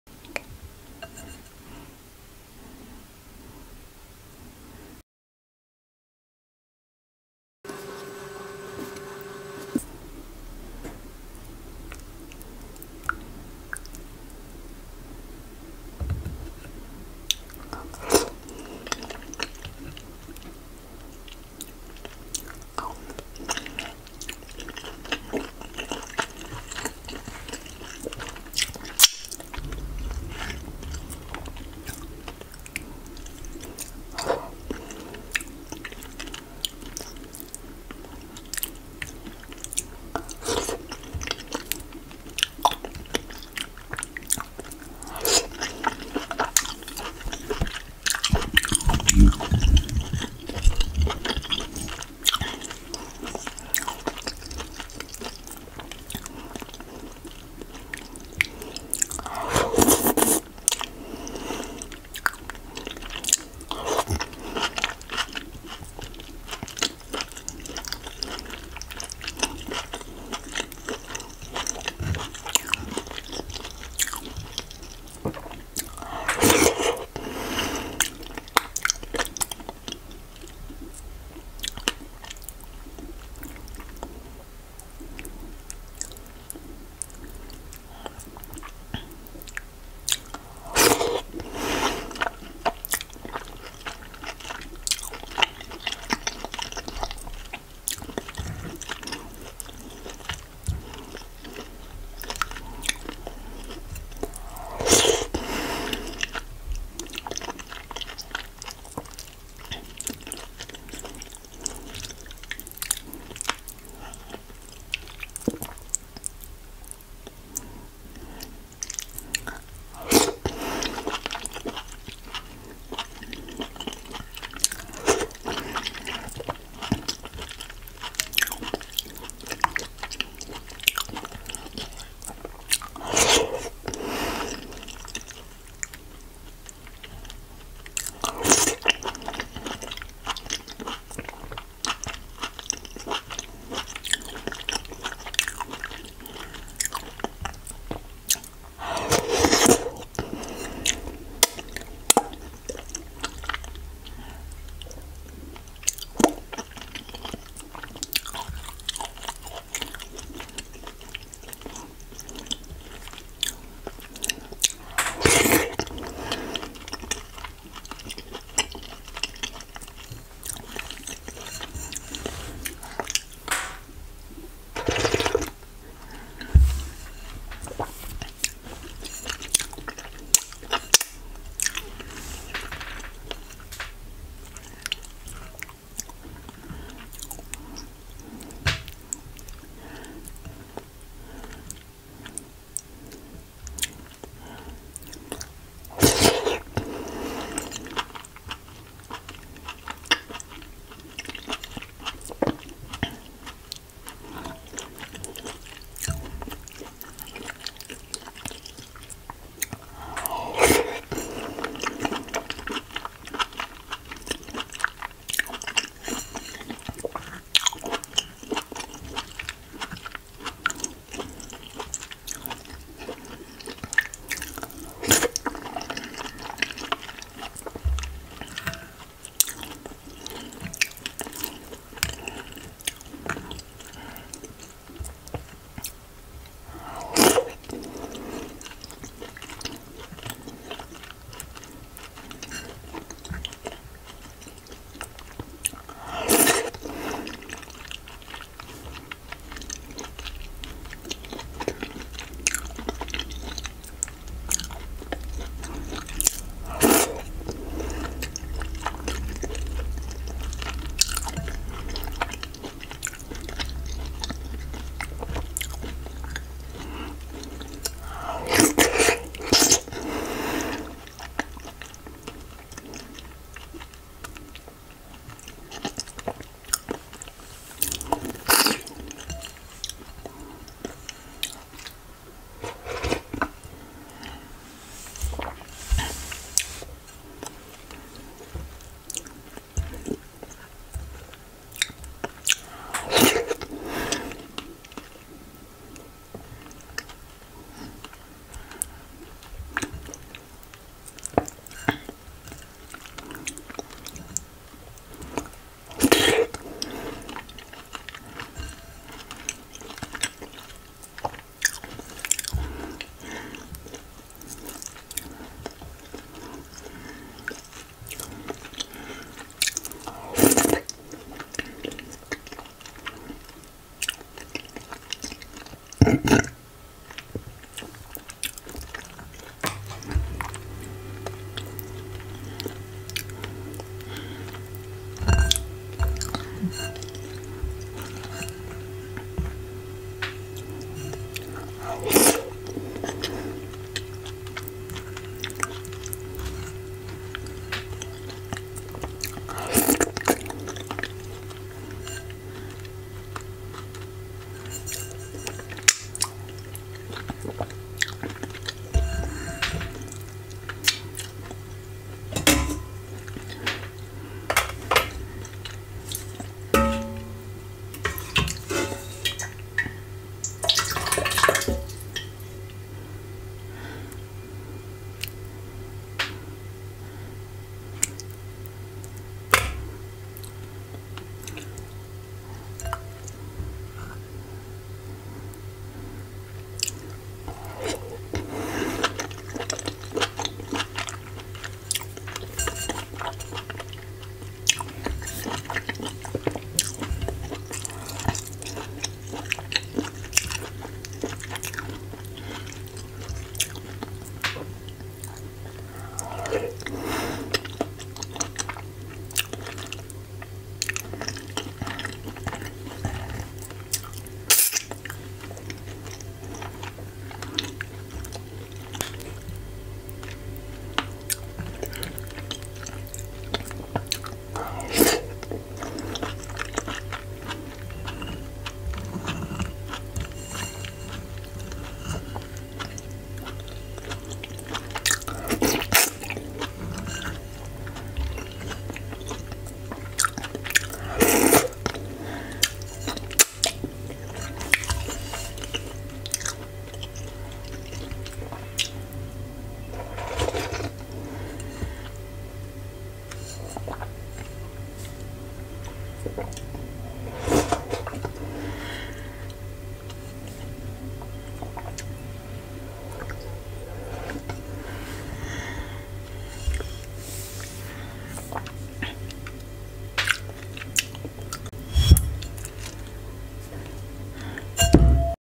ASMR: AFRICAN FUFU MUKBANG//SLURPY SOUNDS!
Hi Foodies and welcome back. Today i am having west african food fufu with soup and i can assure you this was very delicious. Thanks.